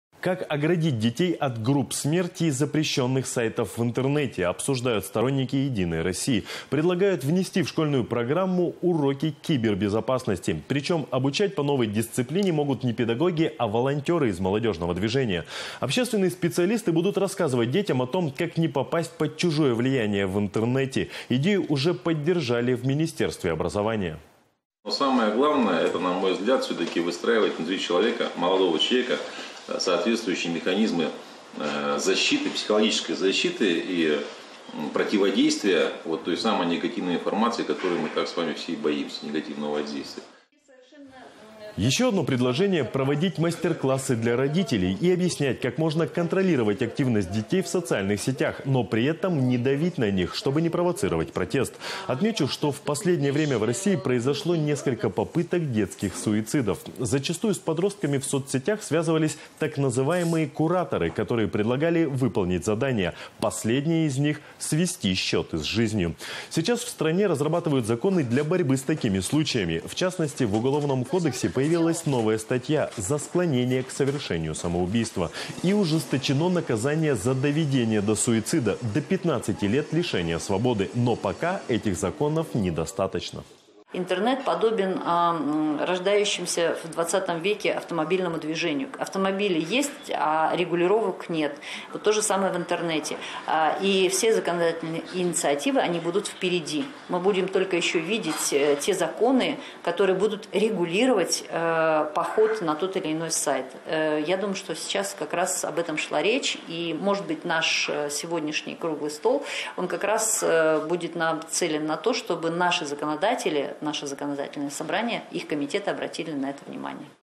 В школах могут ввести уроки кибербезопасности
Как оградить детей от «групп смерти» и запрещенных сайтов в интернете, обсуждают сторонники «Единой России». Предлагают внести в школьную программу «уроки кибербезопасности». Причем обучать по новой дисциплине могут не педагоги, а волонтеры из молодежного движения. Общественные специалисты будут рассказывать детям о том, как не попасть под чужое влияние в сети. Идею уже поддержали в министерстве образования.

Еще одно предложение — проводить мастер-классы для родителей. Но при этом не давить на них. Чтобы не провоцировать протест. Отмечу, что в последнее время в России произошло несколько попыток детских суицидов. Последнее из них — свести счеты с жизнью. Сейчас в стране разрабатывают законы для борьбы с такими случаями. В частности, в уголовном кодексе появилась новая статья — за склонение к совершению самоубийства. И ужесточено наказание за доведение до суицида — до 15 лет лишения свободы. Но пока этих законов не достаточно.